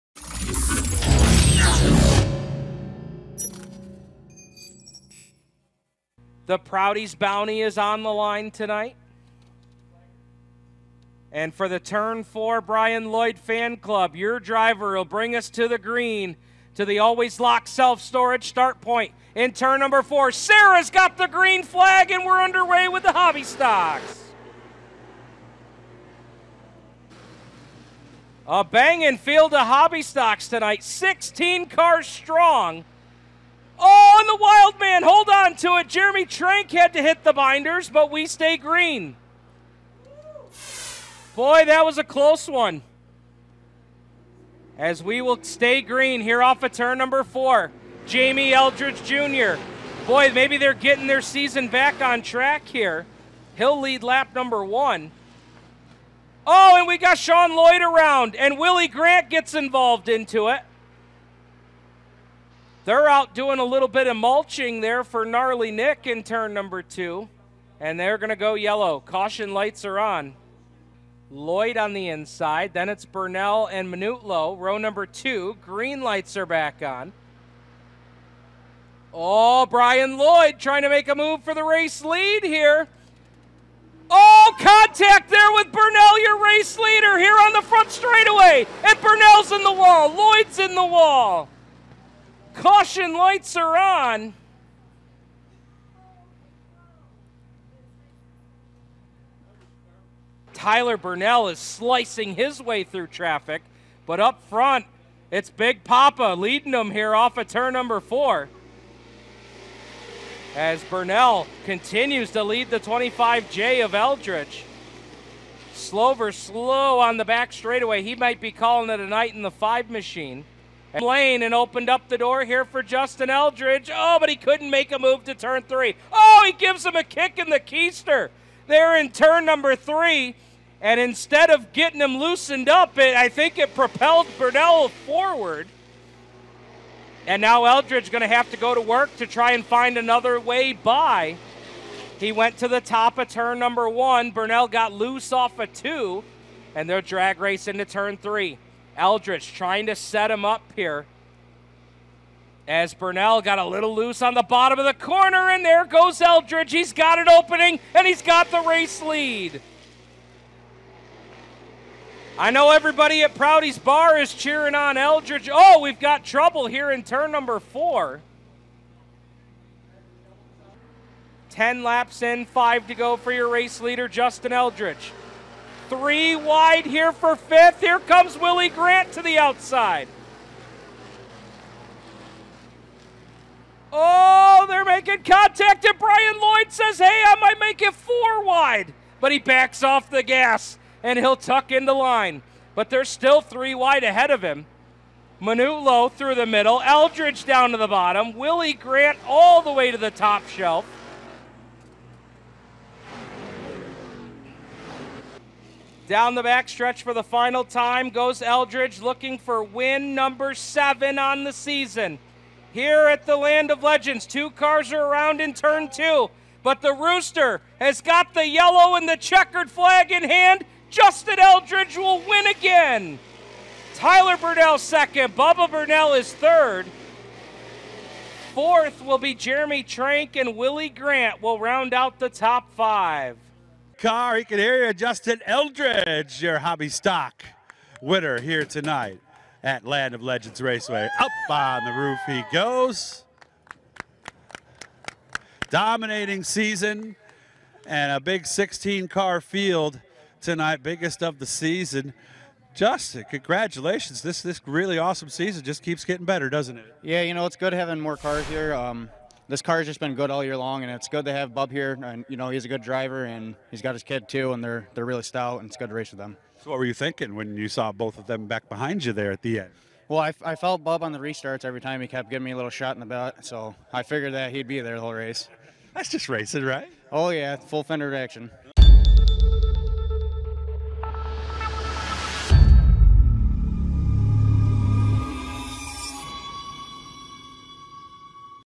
Lloyd's Contracting Hobby Stocks Highlights August 14
In a rough and tumble race, Justin Eldredge locked in win #7 on the year and prevented all of his competitors from winning the Prouty's Bounty for another week. Enjoy these highlights from the Lloyd's Contracting Hobby Stocks!